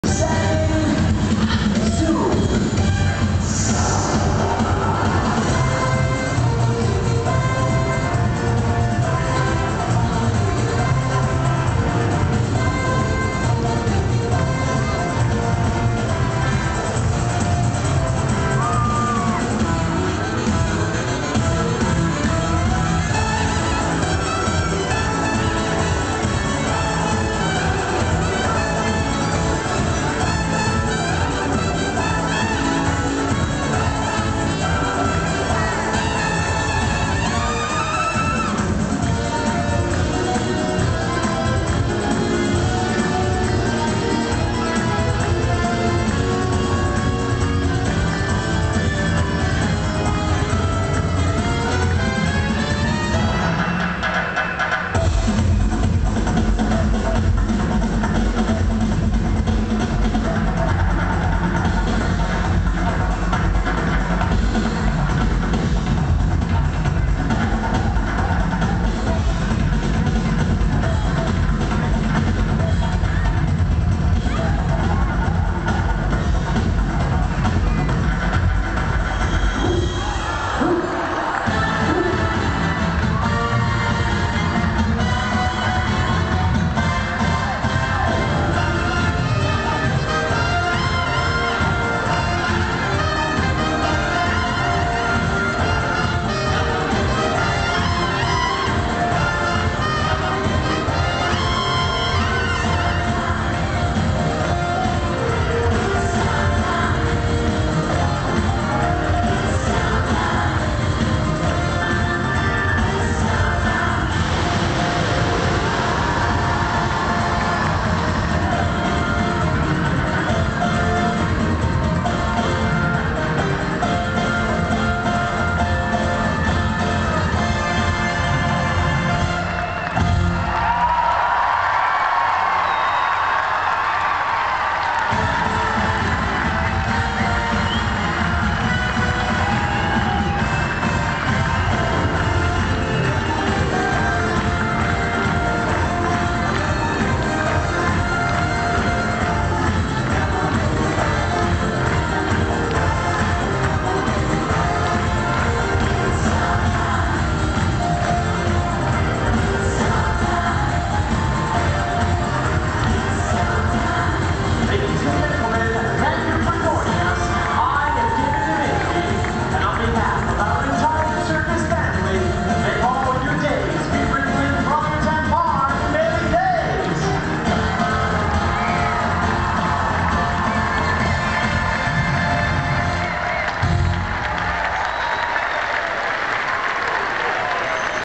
Flighted Camelot Macaws | Greatest Show on Earth
Our camelot macaws performing in the Greatest Show on Earth.